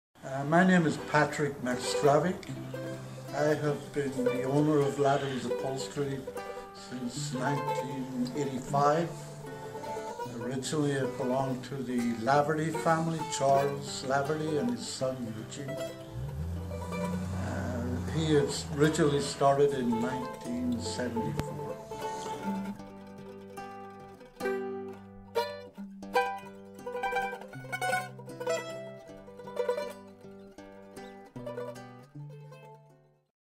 Patrick McStravick loves bringing furniture to life
In 1985, Patrick McStravick purchased Laverty's Upholstry in Richmond and continues to restore furniture and provide related services at the shop at 767-23rd St.